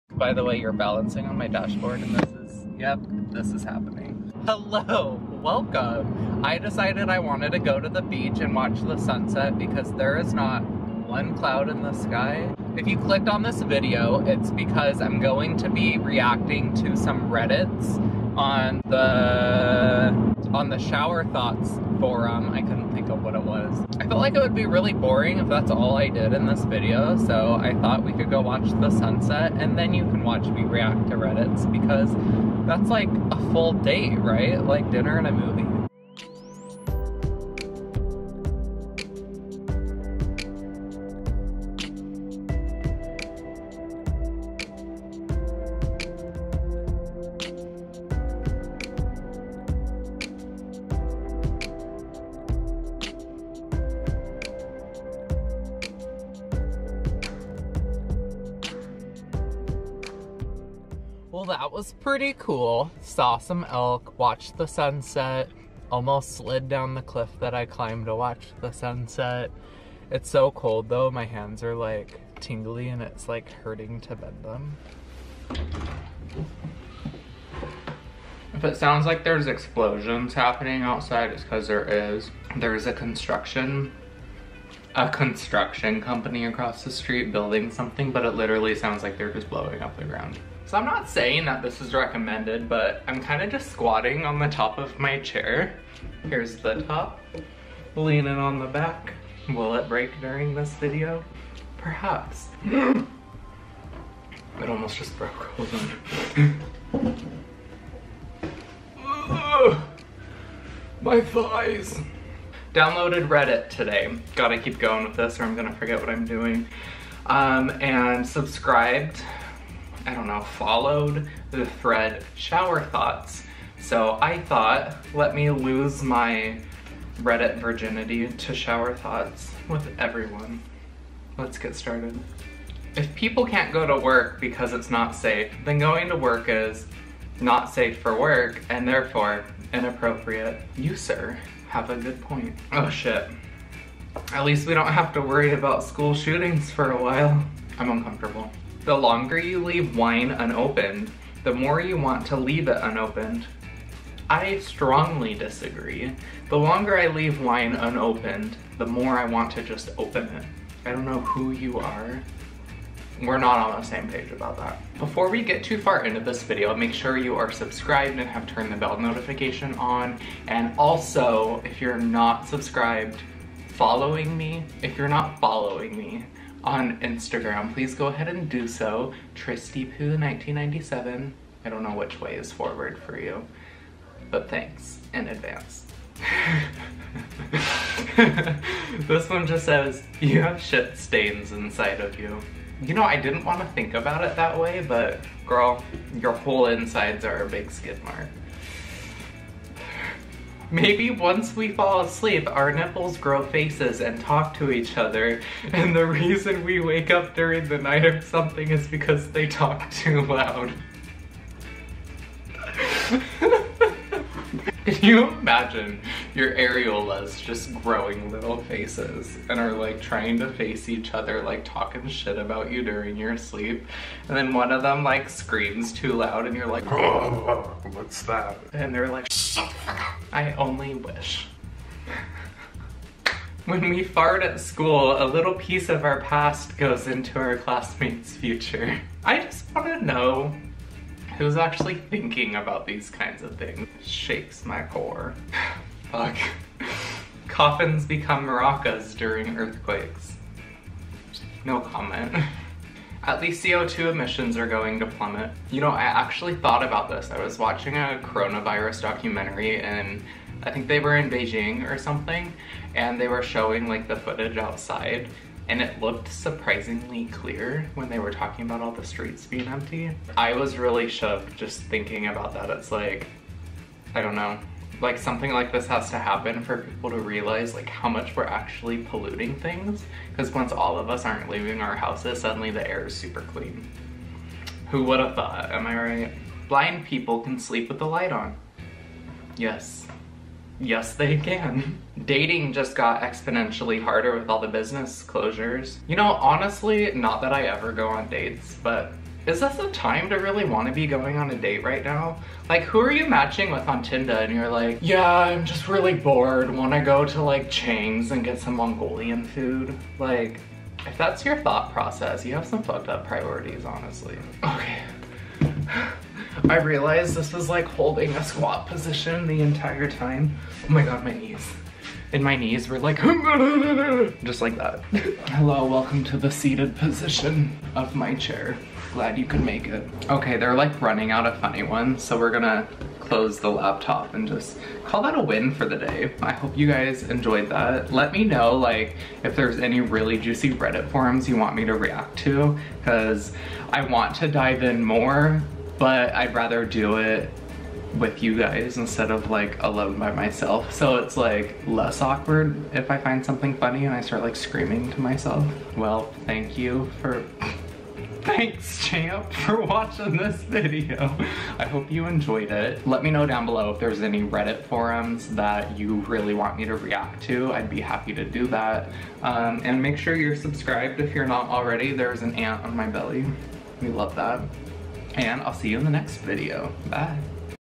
REACTING TO r/ShowerThoughts
This video begins with a little trip to the beach to watch the sun from a mildly unstable cliff. After the beach I dive into the Reddit forum Shower Thoughts and share the one that gave me the most hoots possible.
🌺 Production Info 🌺
Film: Canon G7X Mark ii
Edit: iMovie